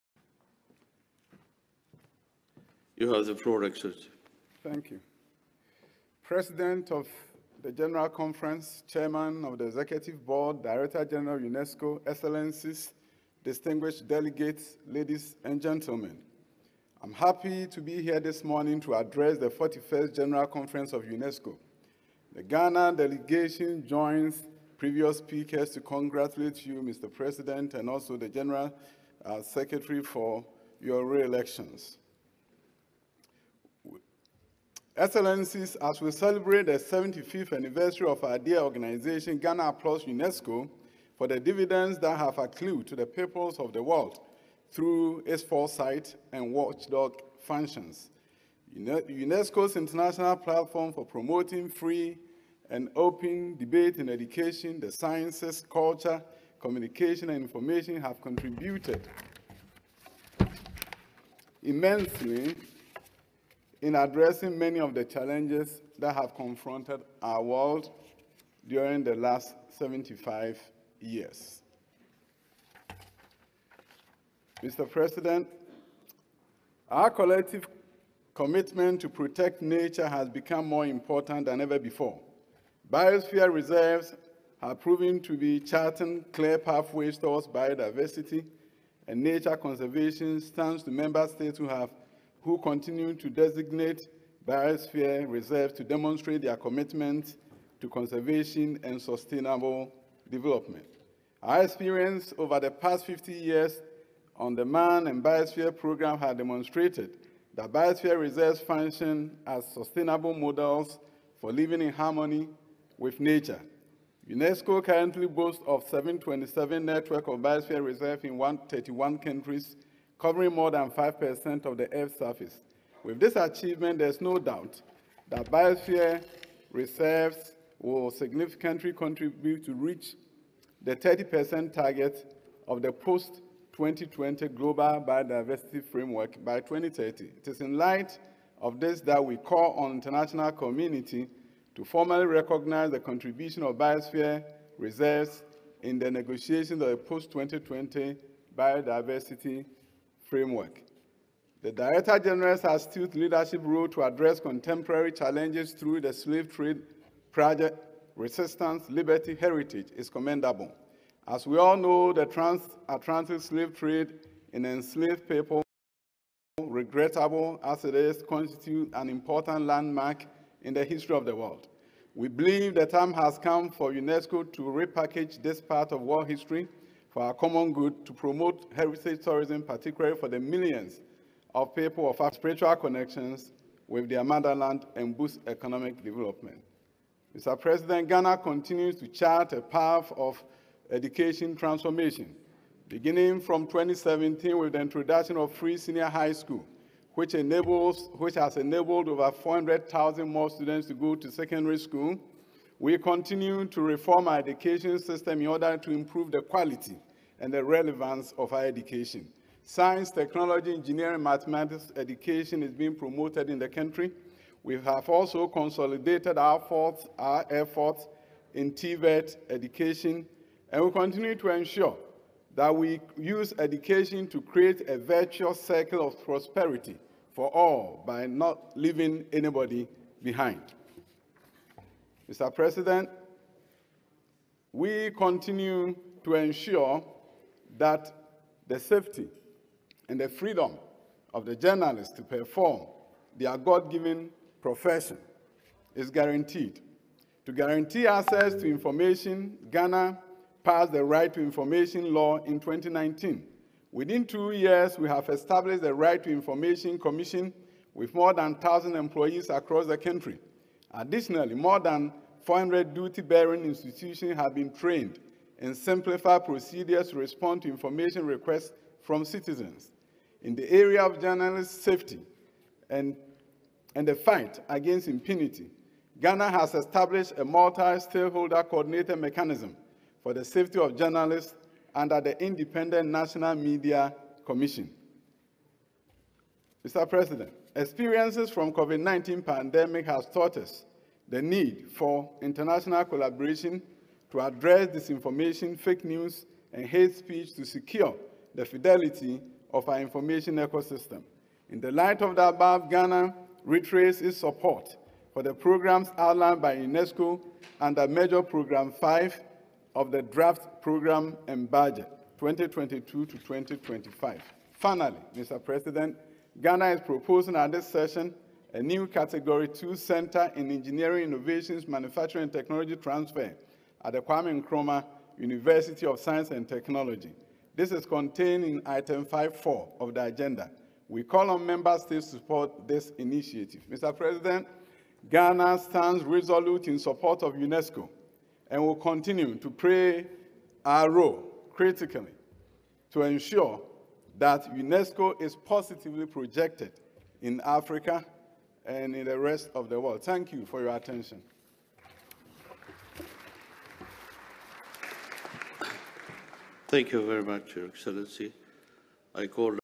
SPEECH BY THE HON. MINISTER FOR EDUCATION DR. YAW OSEI ADUTWUM
Hon. Minister for Education Dr. Yaw Osei Adutwum giving his speech at the 41st General Conference at UNESCO, France HQ.